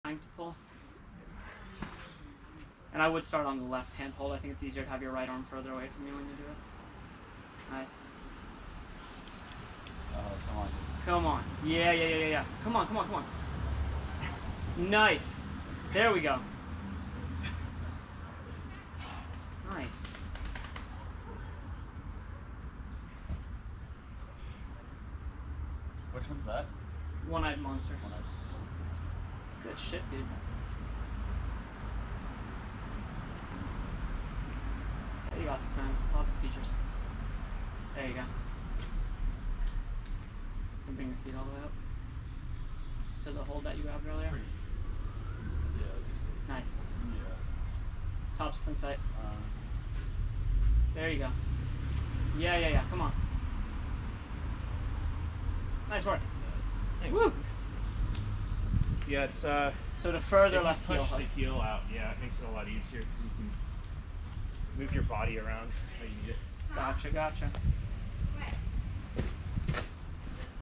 One Eyed Monster (V5) - Smuggler's Notch, VT
Took a bit to figure out the start, key for me was to put the heel further away than I thought so I could transfer more weight over my foot to switch the right hand to extend my arm.